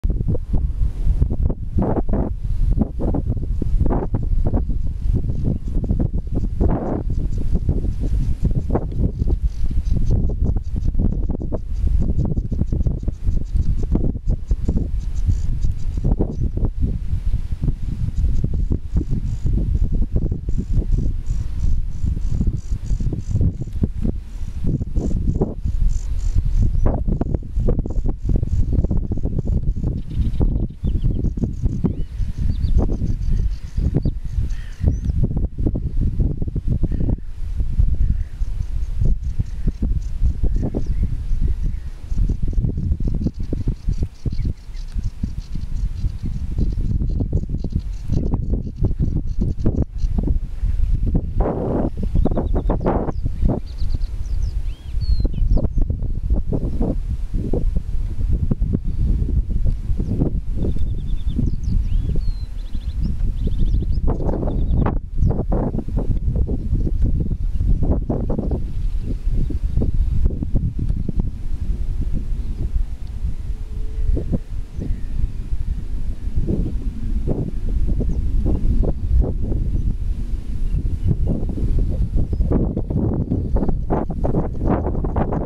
Rózsás flamingó (Phoenicopterus roseus) táplálkozik
Rózsás flamingó (Phoenicopterus roseus) táplálkozik a szabadszállási Zab-széken. 2016. május 28.